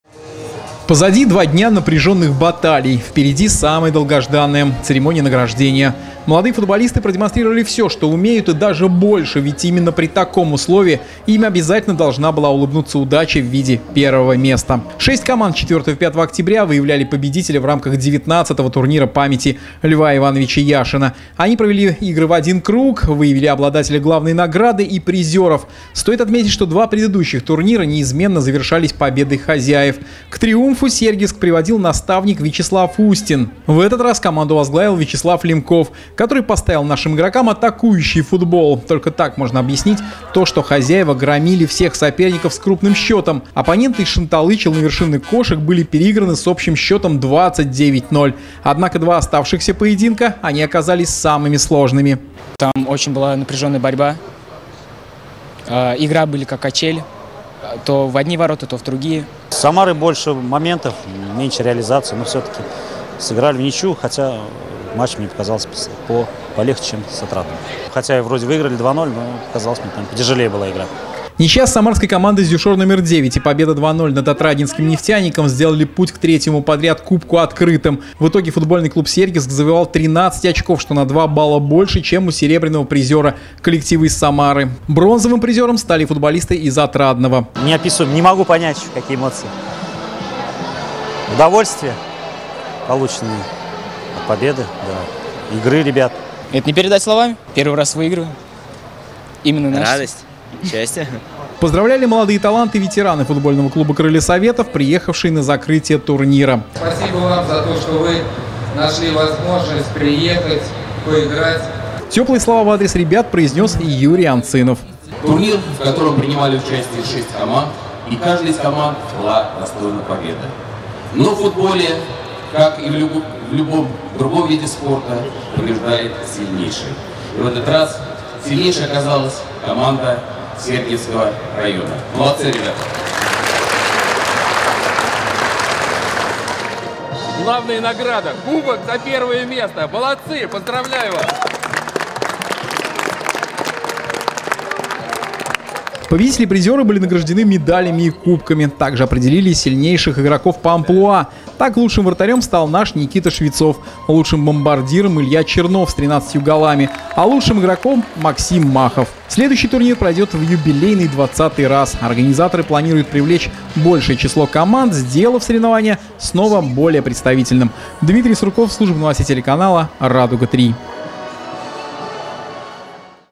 Награждение победителей Кубка Яшина-2018
Ещё одной победой Сергиевского футбола завершился очередной, 19-й по счёту турнир памяти Льва Яшина, который проходил на прошлой неделе в Суходоле.
В течение двух дней борьбу за победу вели шесть юношеских команд, представляющих Самарскую область. Церемония закрытия прошла в пятницу под куполом «Олимпа».